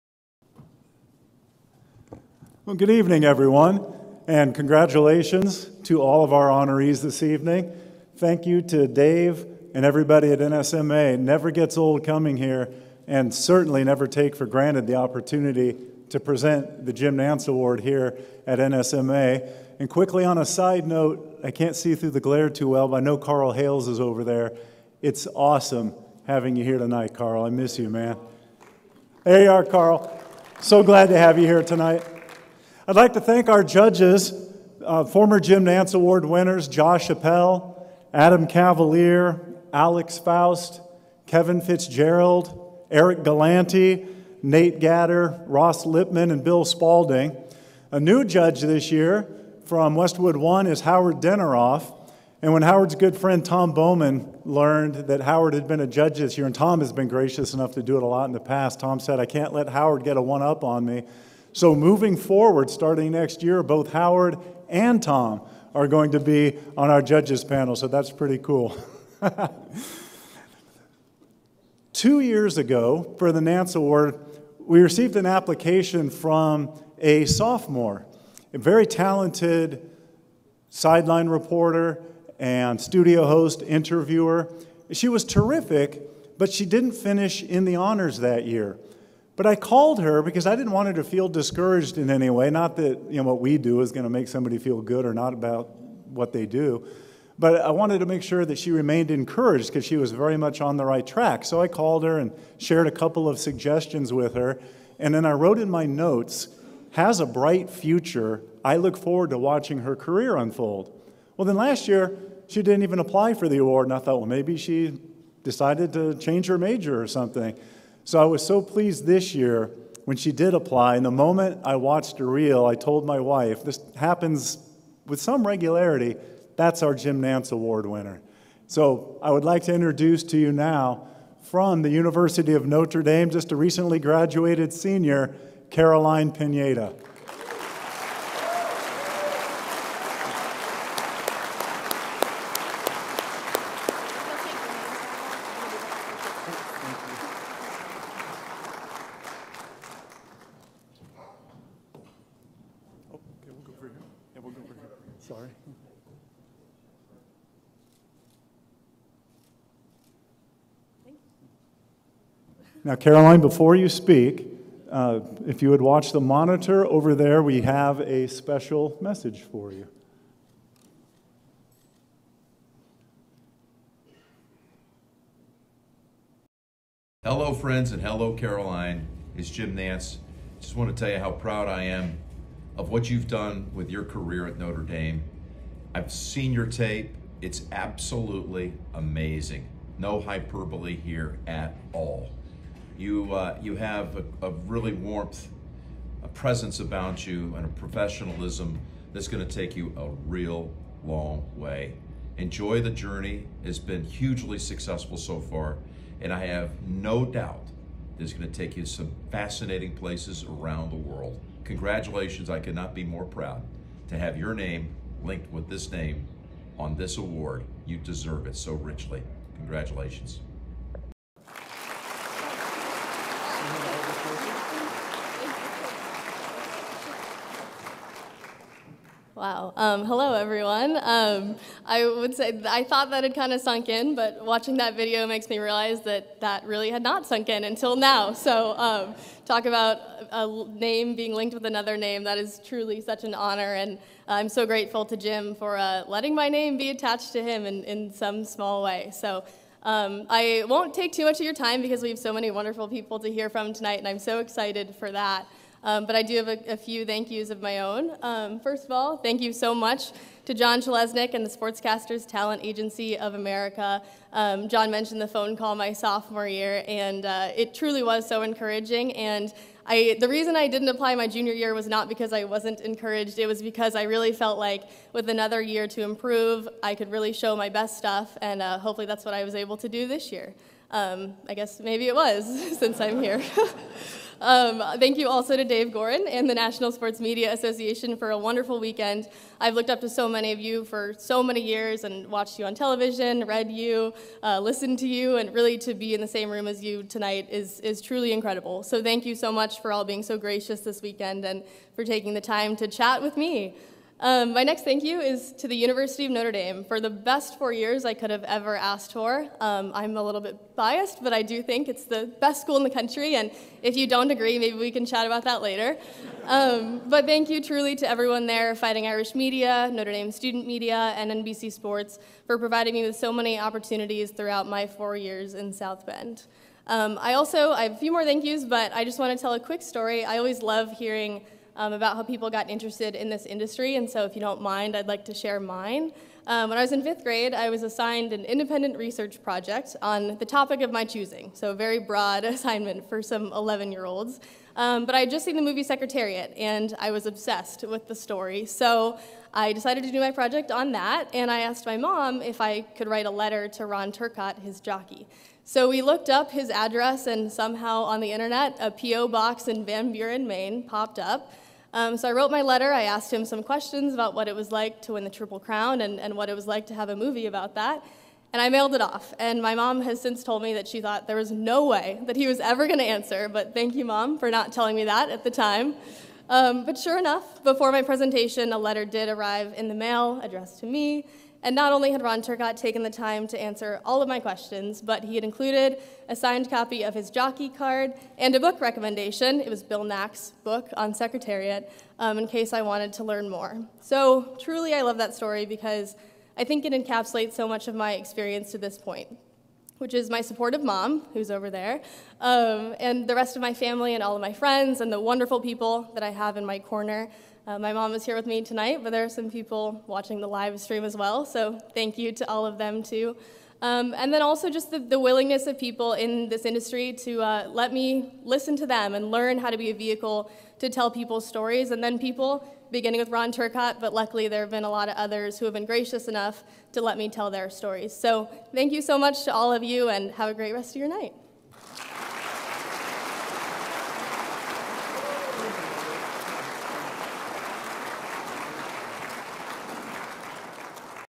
2022 Jim Nantz Award: Caroline Pineda NSMA Award Presentation
Caroline Pineda, the winner of the 14th annual Jim Nantz Award, accepts the award and gives a speech at the 2022 NSMA awards banquet for being named the the nation’s most outstanding collegiate sportscaster.